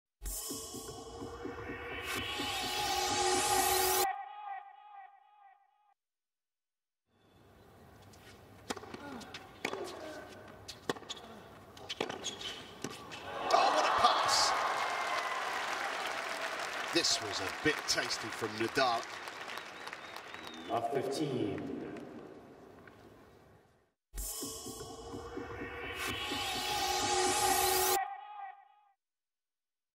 Nadal Rifles Forehand Hot Shot Past Sock
Rafael Nadal comes up with a fearsome forehand hot shot past Jack Sock on Friday in Beijing.